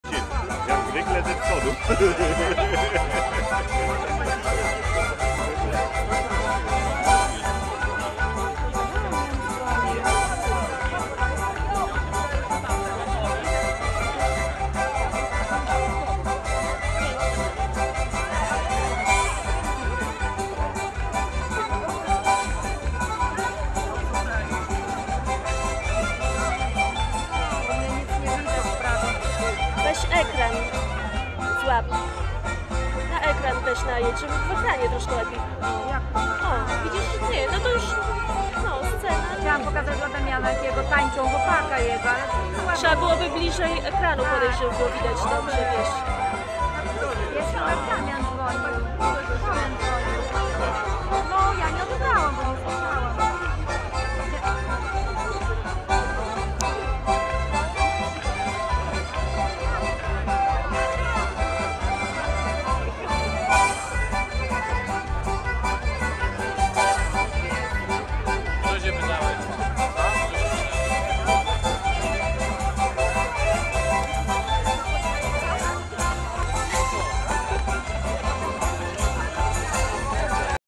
"NA IWANA NA KUPALA" 16 lipiec 2016 - Dubicze Cerkiewne
Taniec Hopak w wykonaniu zespołu "Ranok" z Bielska Podlaskiego